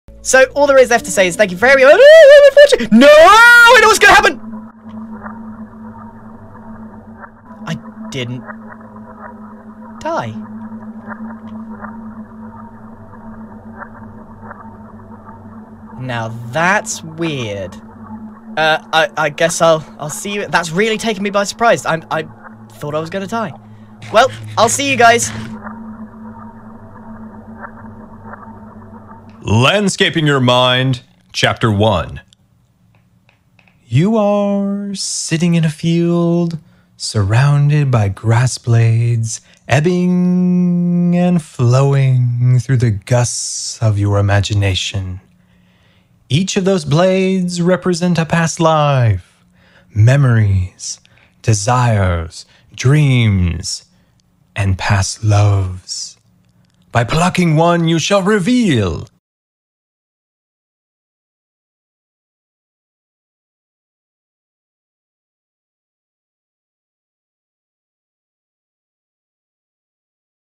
Landscaping Your Mind || Hermitcraft Season 9 Animatic
This one is a little weird...
Idk I wanted to get out of my comfort zone a little, trying to incorporate some more fun angles and push poses.

Does it make any sense? Not really.
But I guess now it just fits into Grian's weird season-

But maybe the weirdness was just an excuse so I could put Techno in a Hermitcraft Animatic. :P

TW some flashing images!

This was done just for fun and to practice animatics/storyboards.

Last but not least, the awkward tagging section:
Grian MumboJumbo GoodTimesWithScar Animatic Hermitcraft Season 9 Minecraft Championship Evo SMP